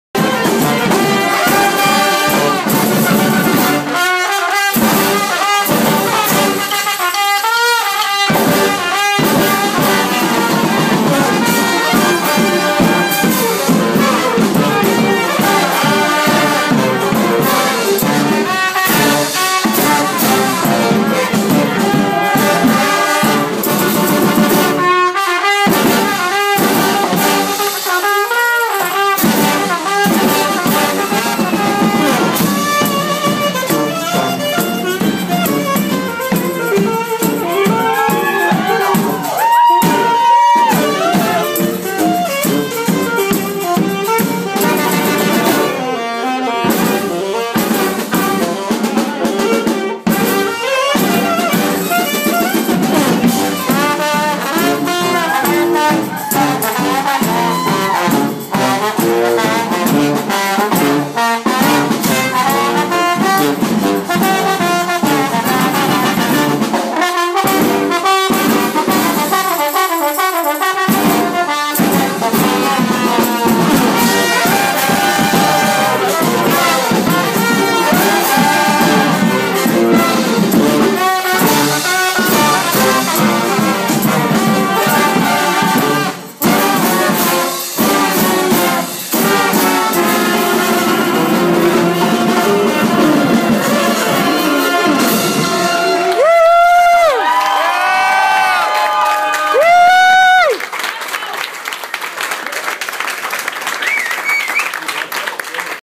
Detroit Party Marching Band Invades IMG8012
Detroit Party Marching Band Invades Grand Rapids. First time in the city. Event at the DAAC covered in RapidGrowthMedia.com May 2013.